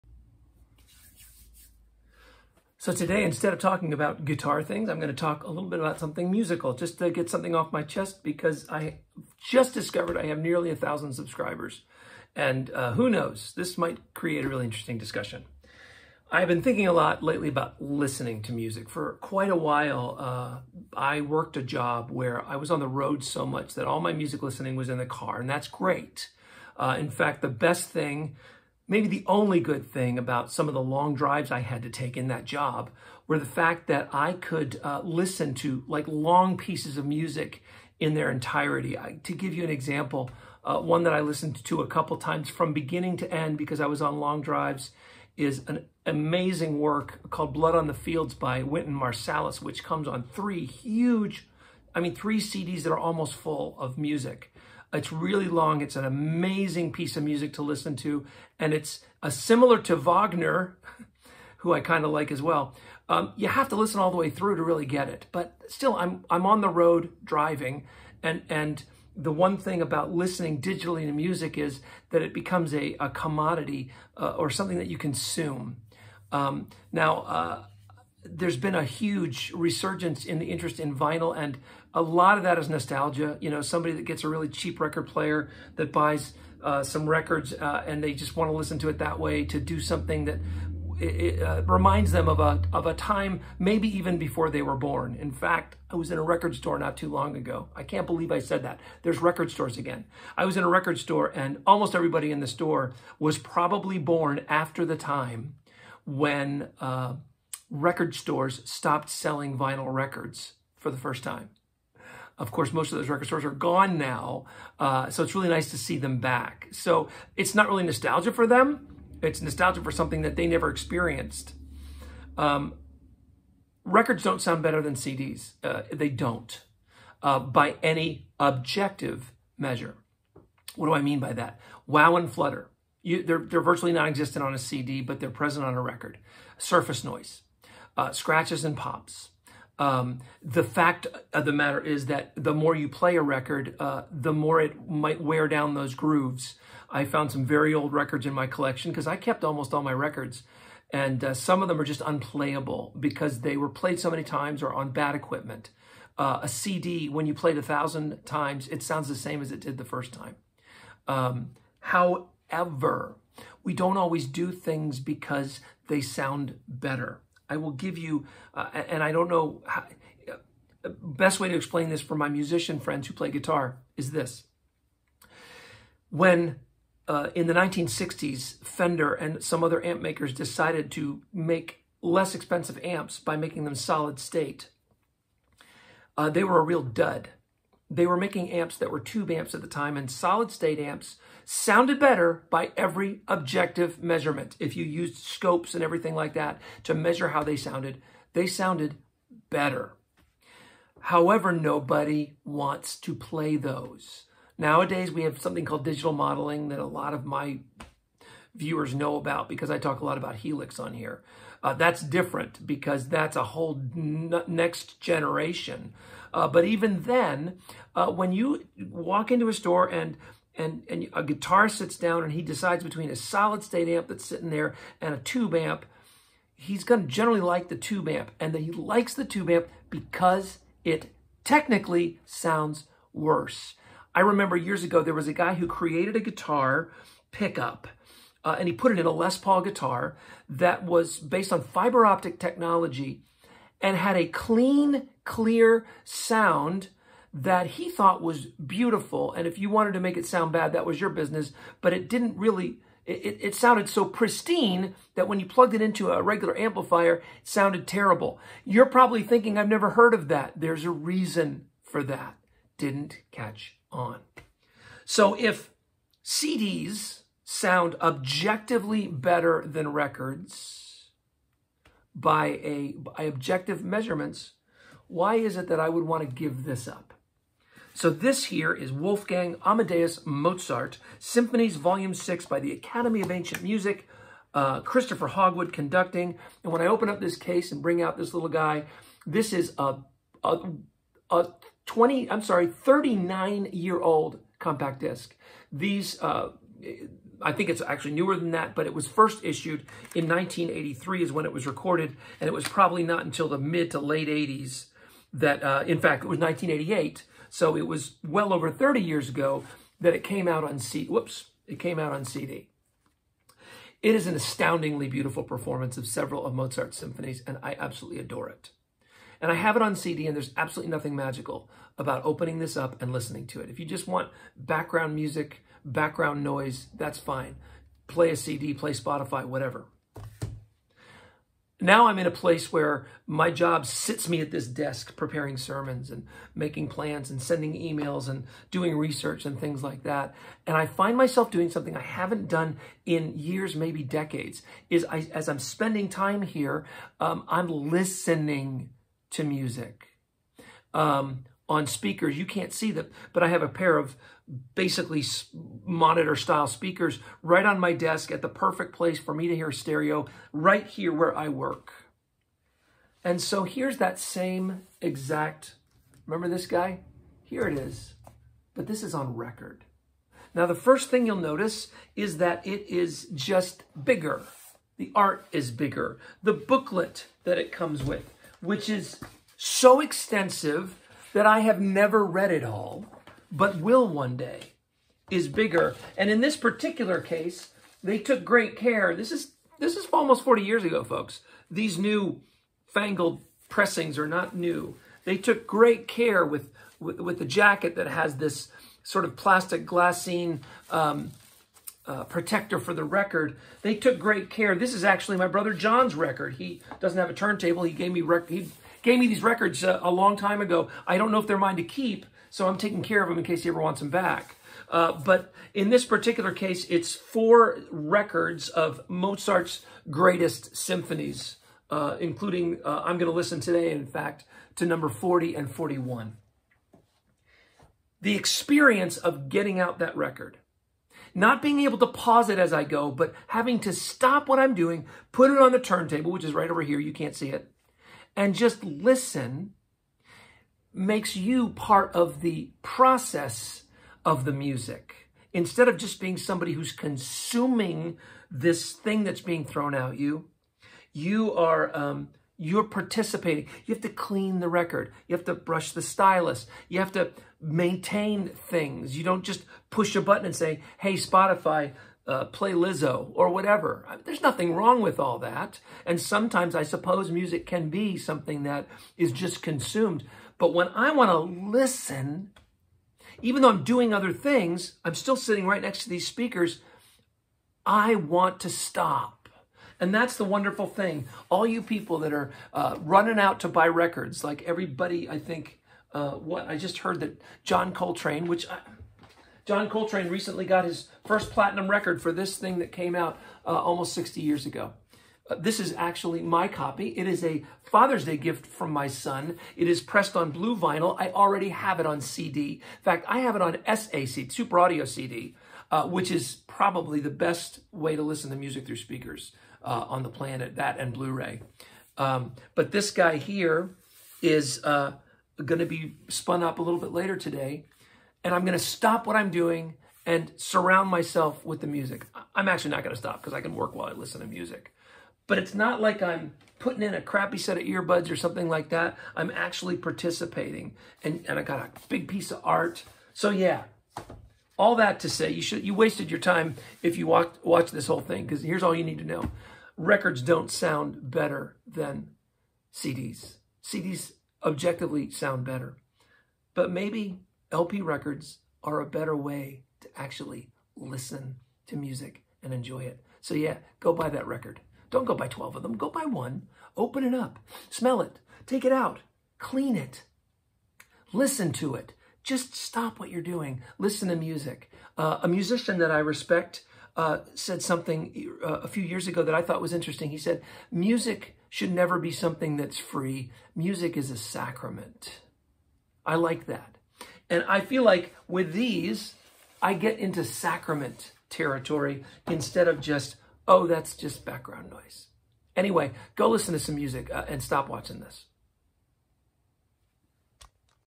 How to LISTEN to Music
CDs sound better than vinyl. You can prove that objectively with measuring equipment... So... why am I listening to SO many records these days!?!?!

Yes, CDs sound "better" than records... But better doesn't mean a more pleasing listening experience...